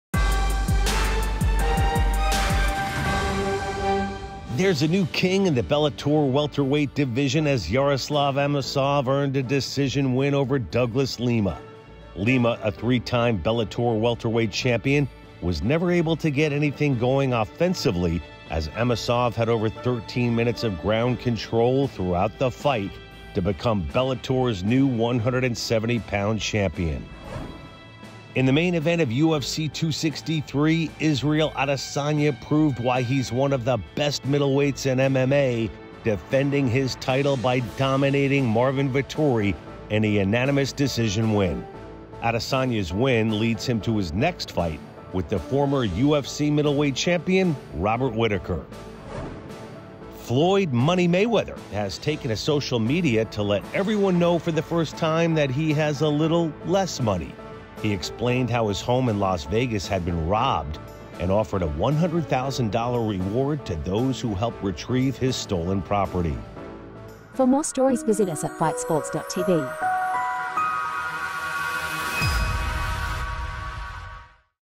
Amosov Wins Bellator Welterweight Title; Adesanya Defends UFC Middleweight Belt| FIGHT SPORTS
Yaroslav Amosov Wins Bellator Welterweight Title over Douglas Lima; Israel Israel Adesanya Defends UFC Middleweight Belt against Marvin Vettori and Floyd Mayweather offers $100,000 reward for information on his Las Vegas home that was burglarized. All of this in todays Fight Sports Daily News.